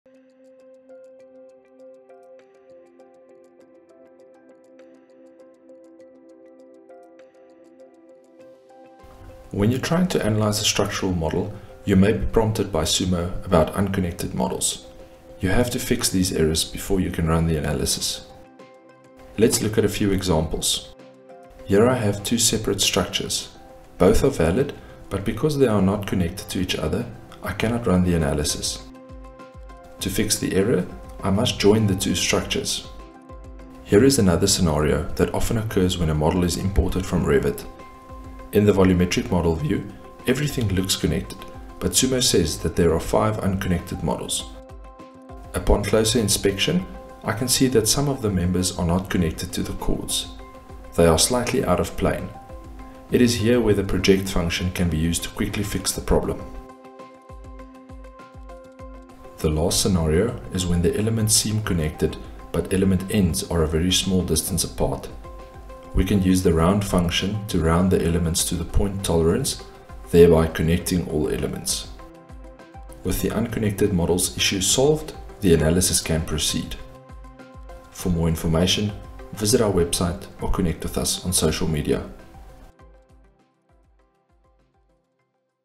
Fixing Unconnected Models in PROKON Sumo
Struggling with unconnected models in PROKON Sumo? Don’t let this common issue stop your structural analysis!

With these quick tips, you’ll be ready to run your analysis smoothly.